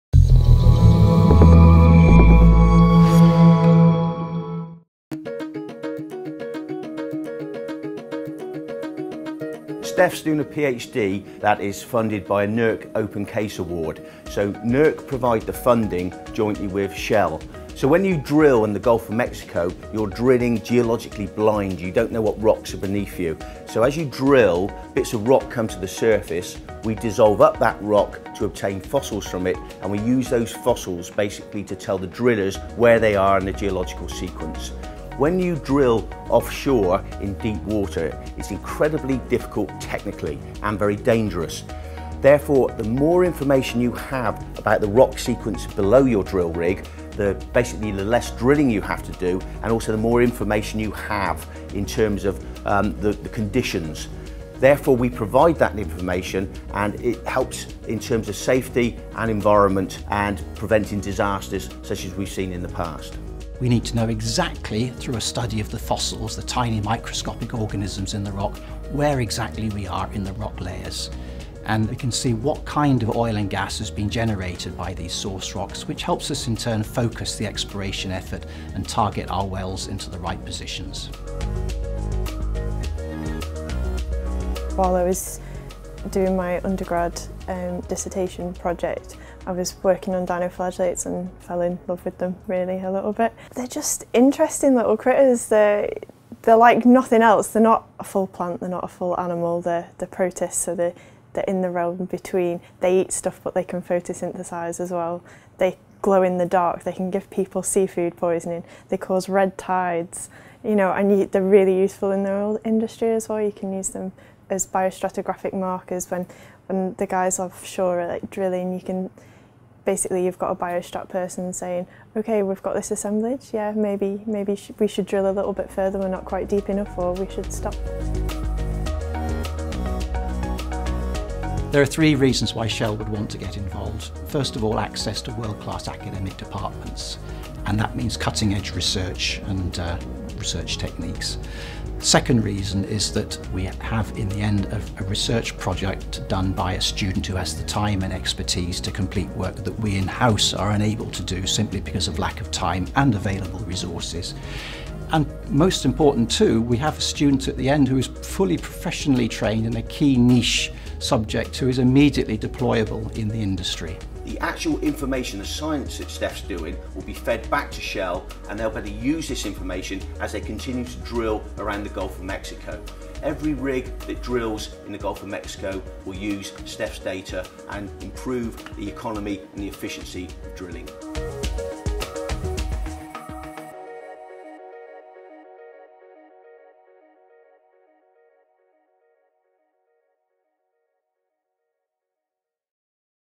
Shell Partnership for Microfossil Analysis
A NERC Open CASE Studentship on the theme of energy has brought together palynology PhD student Stephanie Wood, University of Sheffield, and leading petrochemical company Shell. Together they are investigating the rock strata under the Gulf of Mexico.